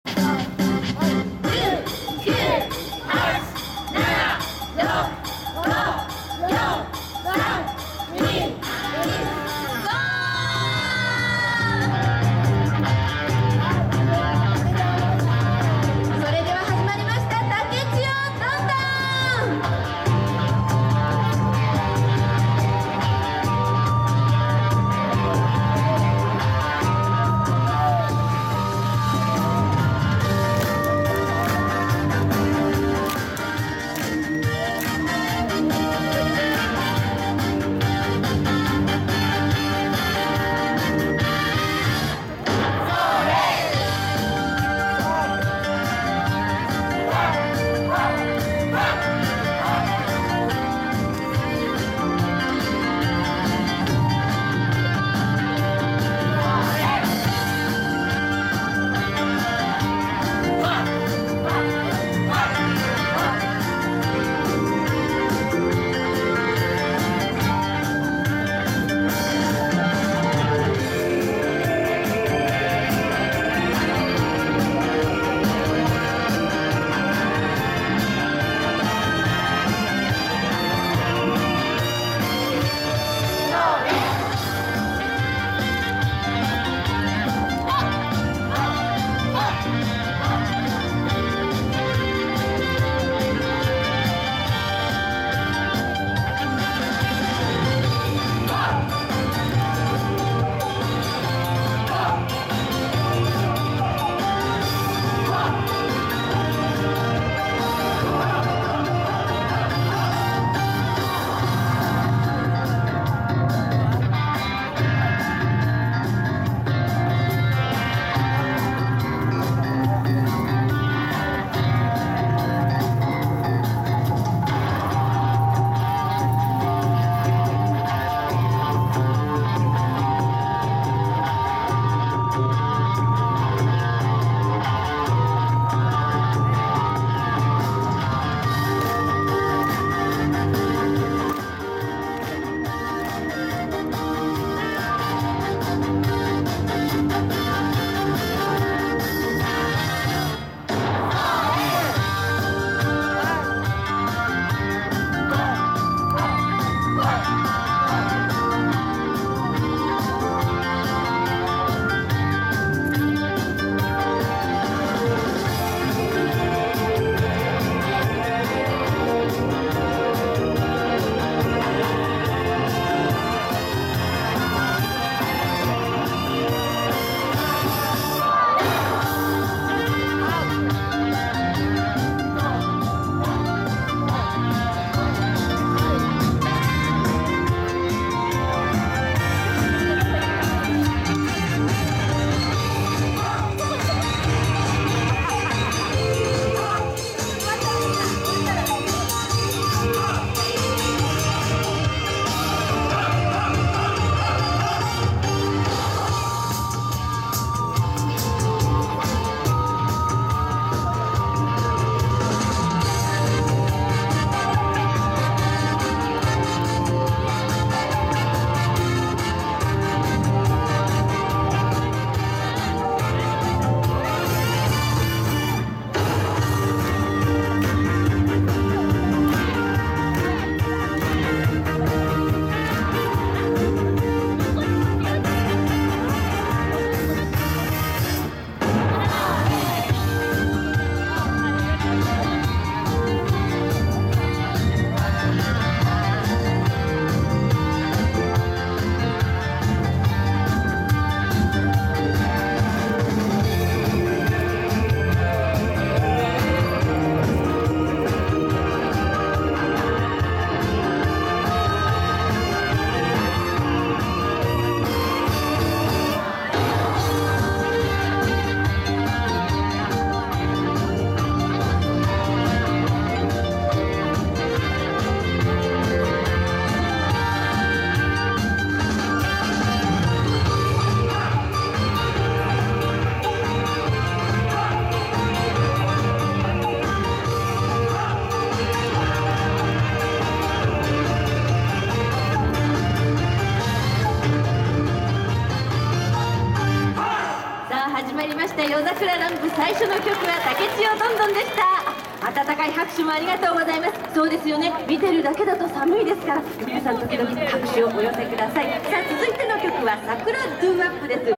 🌹20代🌹美男女と子供と😍着物姿の🌹熟女たちの🌹夜桜乱舞🔥ｵｼｬﾚで🔥ｶﾗﾌﾙな衣装で🔥楽しく🔥踊り💖観客の日常の🔥疲れをいやす🌸静岡祭り🌹kinkakujitv で検索 🌹118
E:\(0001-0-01興味,仕事2021年1月から　主\(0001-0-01興味,仕事2021年4月\()0001-静岡祭2021年令和3年4月3日（土～4月4日（日）と葵舟オープン電話221-1310